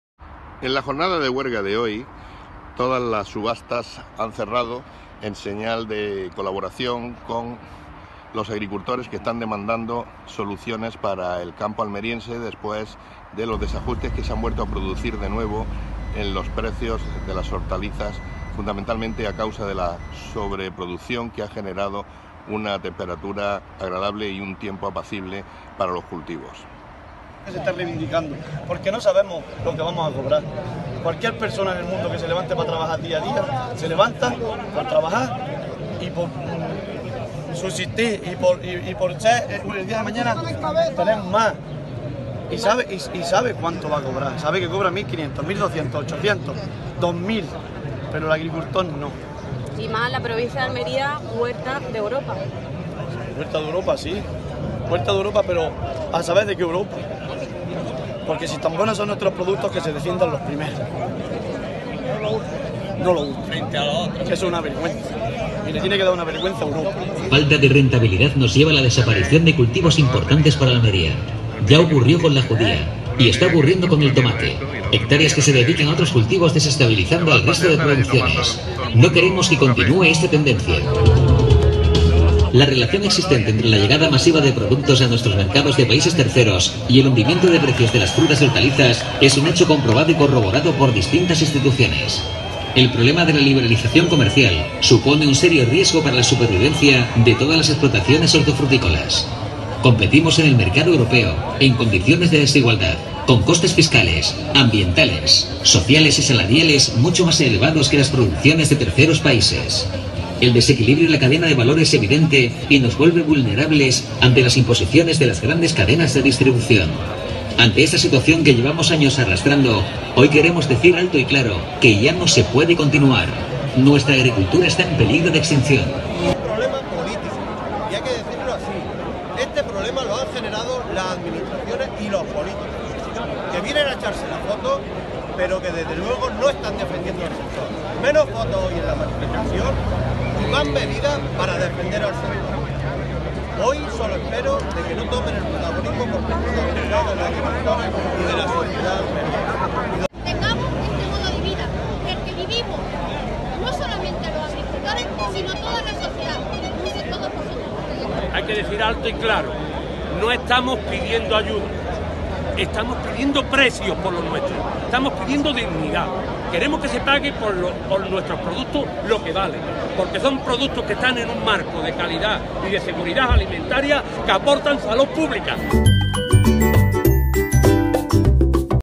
El paro agrario en Almería - 19/11/2019
Los agricultores almerienses se unen para reivindicar los bajos precios de frutas y hortalizas que están sufriendo en los últimos tiempos. Más de 20.000 productores y profesionales del sector se han manifestado ante este crisis de precios en el sector hortofrutícola, exigiendo las medidas para el control de importaciones extracomunitarias, restauración de los certificados de exportación, el control de tasas arancelarias y la revisión de los precios mínimos de entrada de productos extracomunitarios.